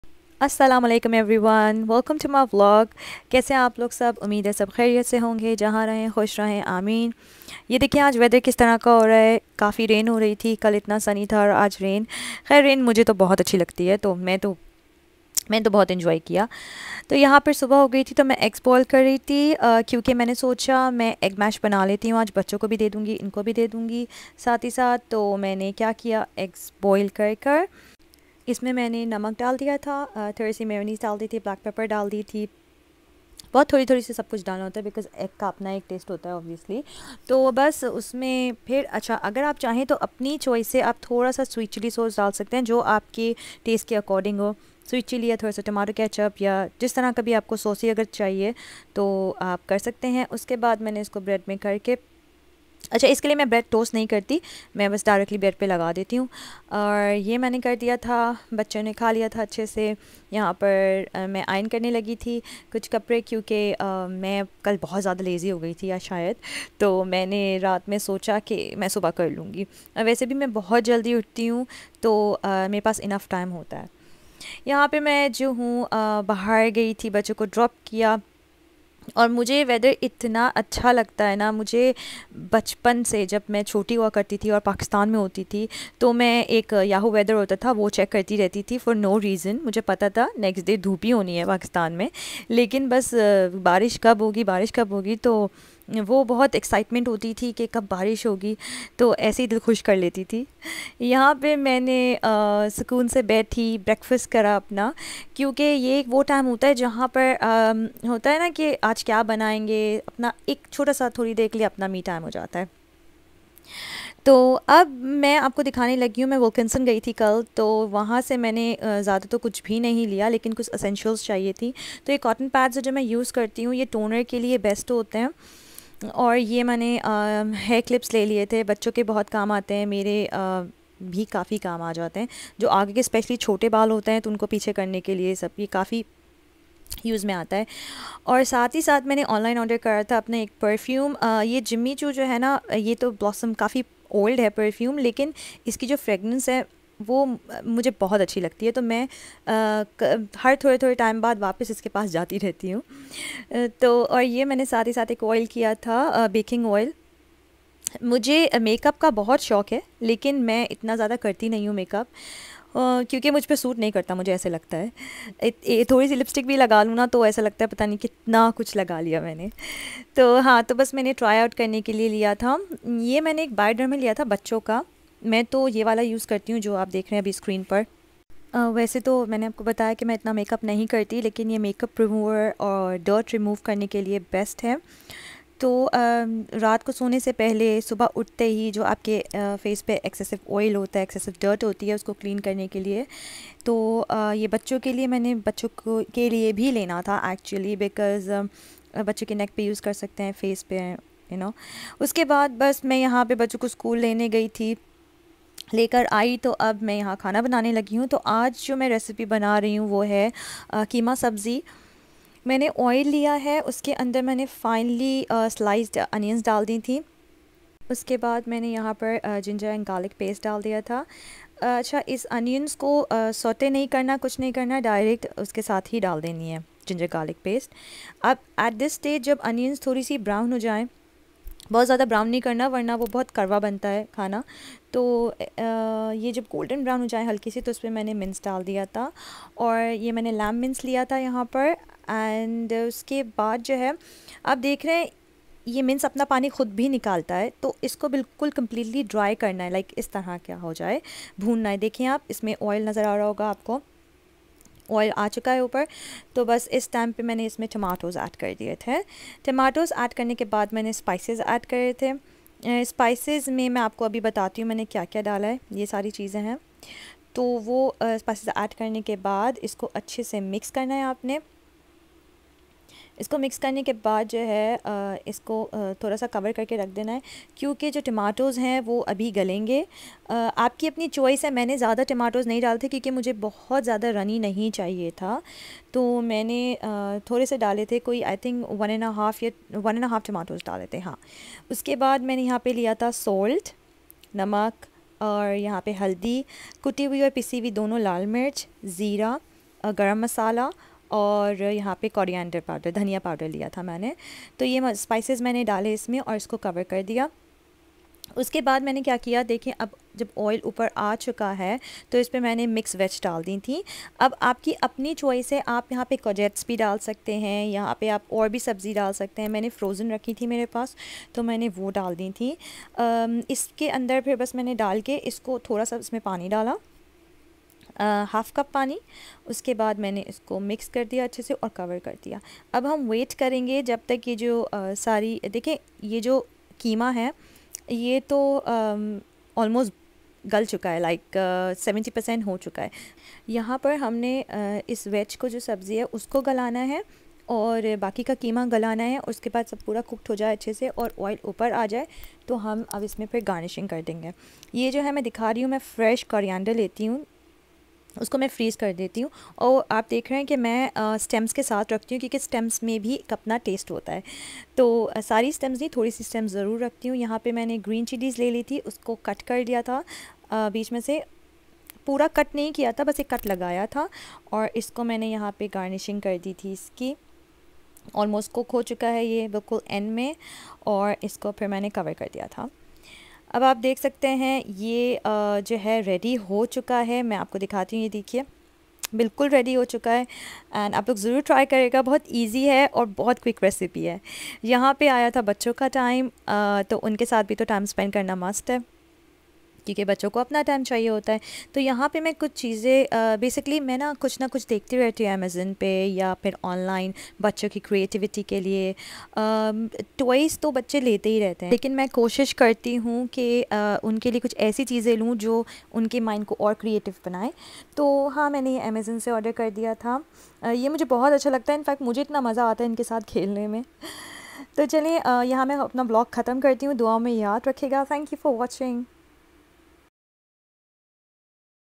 Easy and quick keema sabzi recipe and a quick haul. Huma Vlogs 🇬🇧
Hey Guys!

Enjoy x